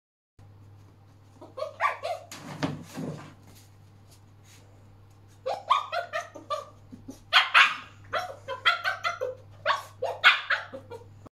Puppy demands more treats | Husky Malamute
Hi I'm Luna I'm a Huskamute Puppy. I decided i really liked my new treats and was running around shouting to demand the humans give me more.

     - Cute photos of me almost daily!